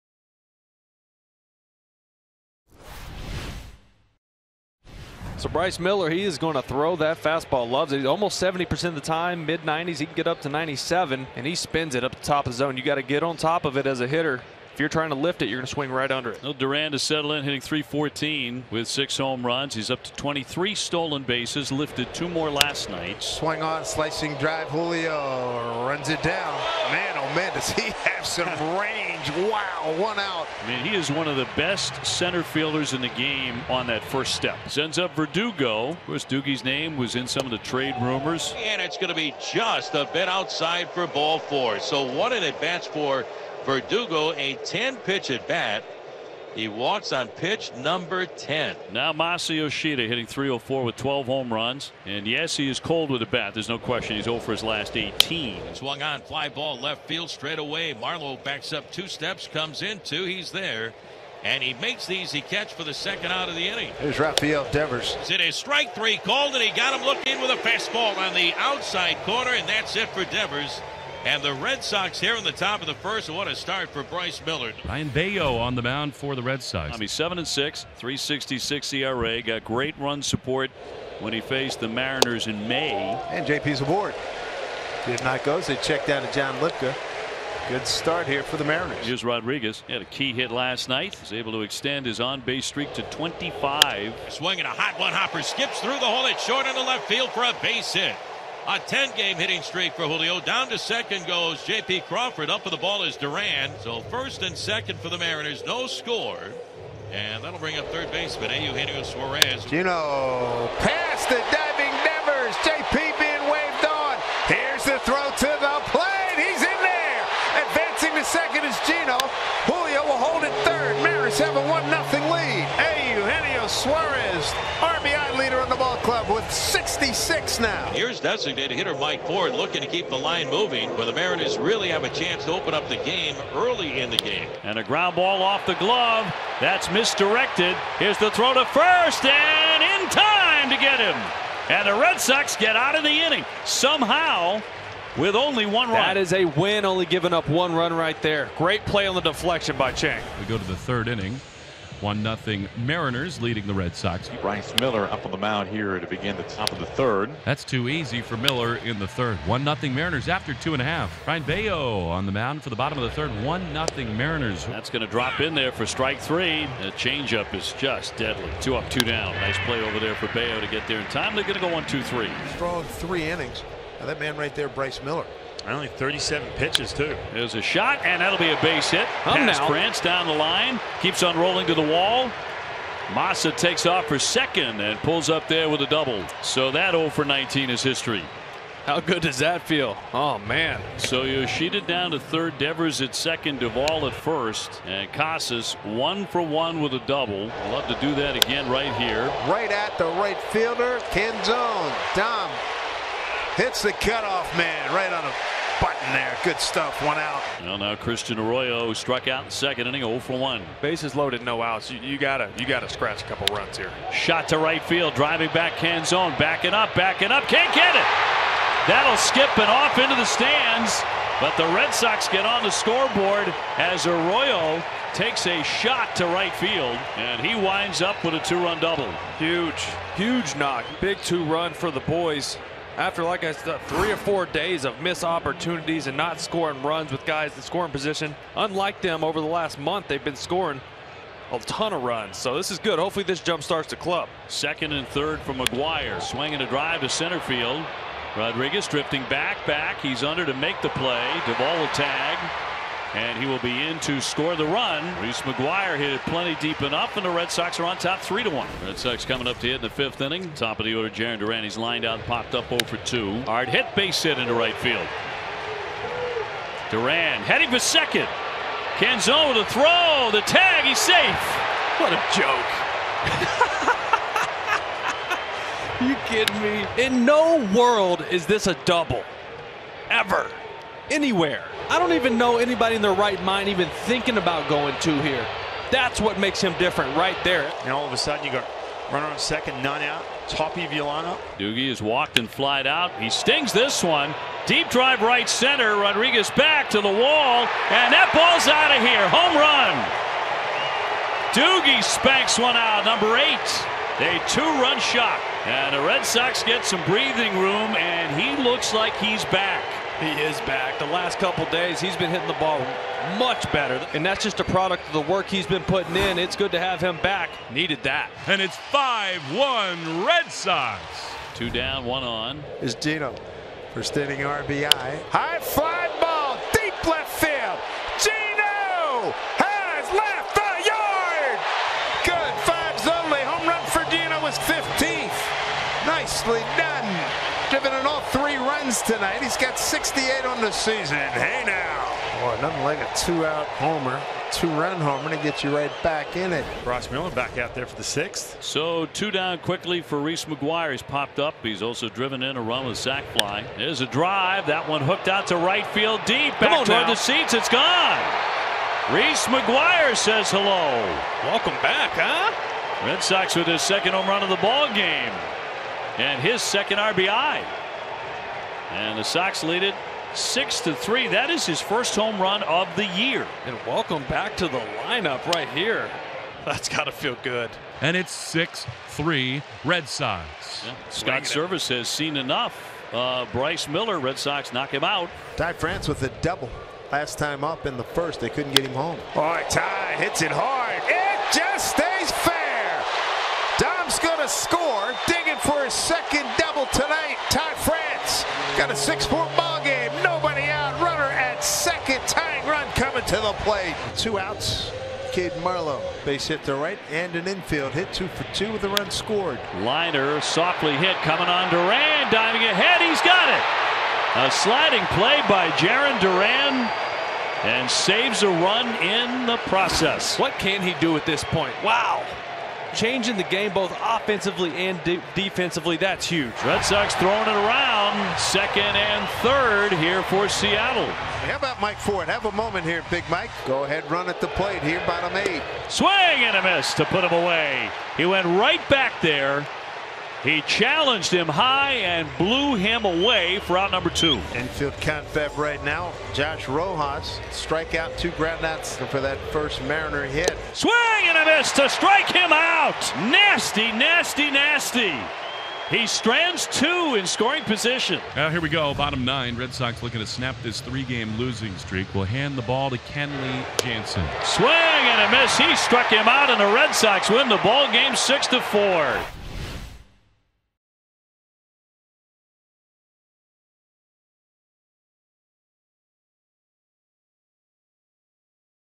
Red Sox vs. Mariners Game Highlights (8/1/23) | MLB Highlights
Red Sox vs. Mariners full game highlights from 8/1/23, presented by @americasnavy